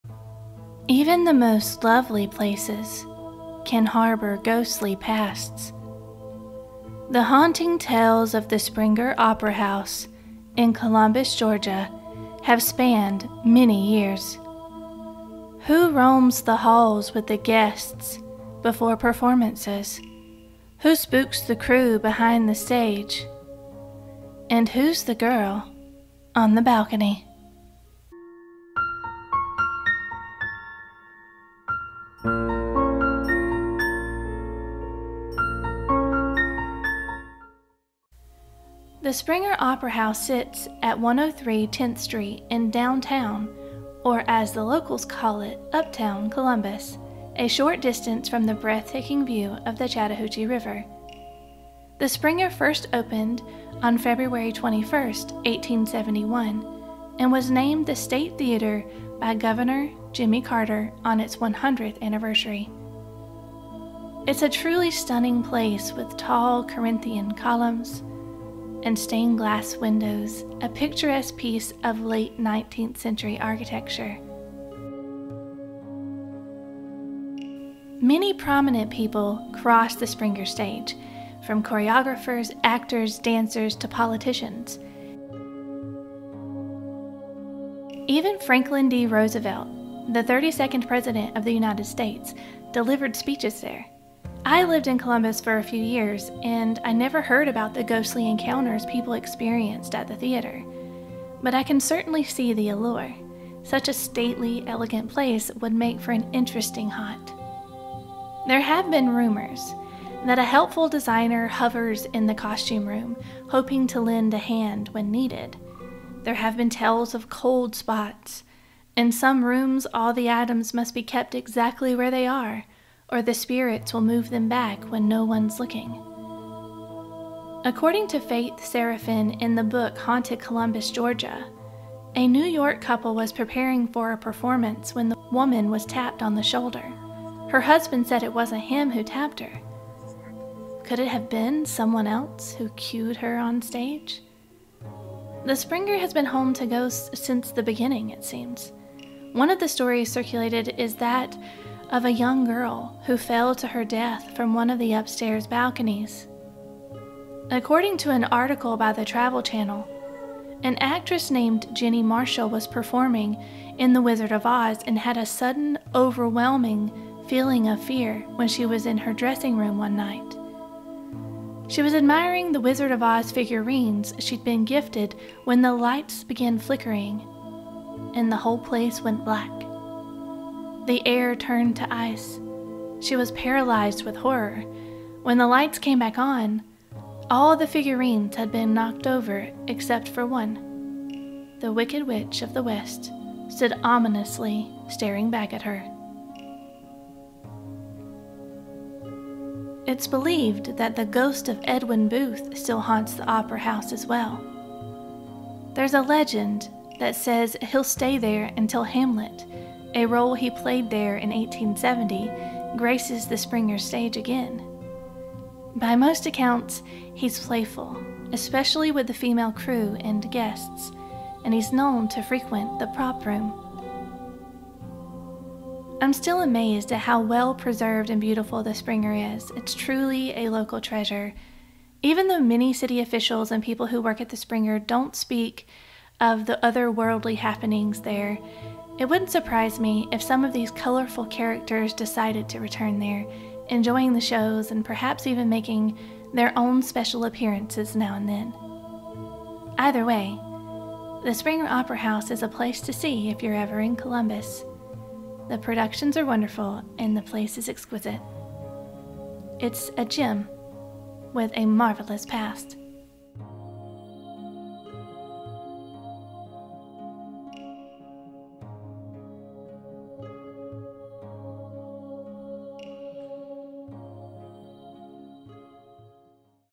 Unveiling the Secrets of the Haunted Springer Opera House
Haunted Springer Opera House in Columbus, GA

The Springer Opera House in Columbus, GA, has been rumored to be haunted for many years. Hear about a few of its ghost in this video. If you'd like to learn more history about the Springer, check out the full episode of the Fabled Collective Podcast

• Video by Jacques Barrette from Pixabay
• Video by Life-Of-Vids from Pixabay
• Lillie Langtry Photo: Mrs. Langtry 1899, Wiki Media Commons, Public Domain
• Candle Video by klimkin from Pixabay
• Door Opening Video by Jacques Barrette from Pixabay
• Smoke Video by Jacques Barrette from Pixabay
• Multiple Candle Video by Jacques Barrette from Pixabay
• Dust Particles Video by Jacques Barrette from Pixabay
• Skull Video by Stefania Buzatu from Pixabay